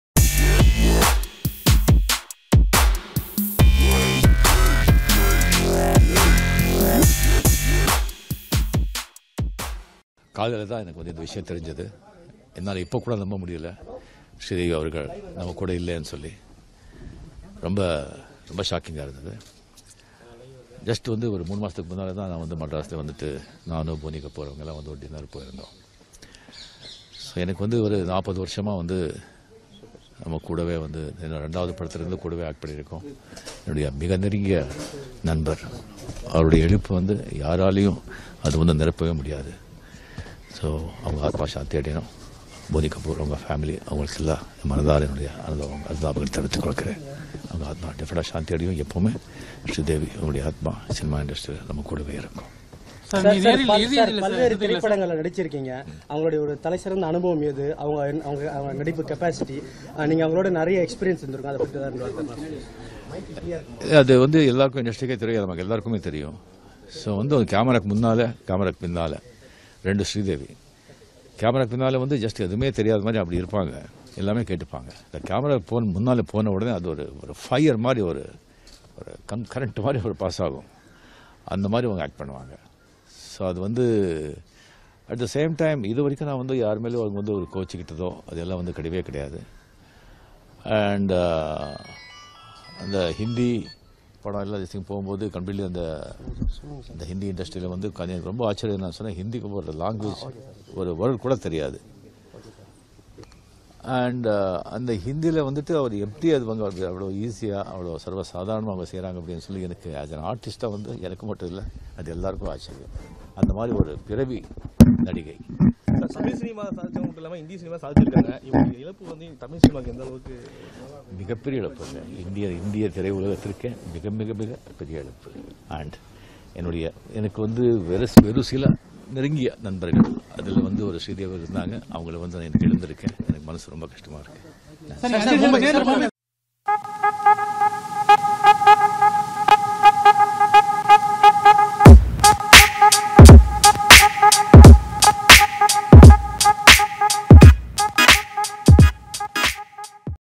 Rajinikanth interview about Actress Sridevi dead | நடிகை ஸ்ரீதேவி குறித்து ரஜனிகாந்த் பேட்டி
Rajinikanth interview about Actress Sridevi dead.

நடிகை ஸ்ரீதேவி குறித்து ரஜனிகாந்த் பேட்டி.

Must Watch

Reena TV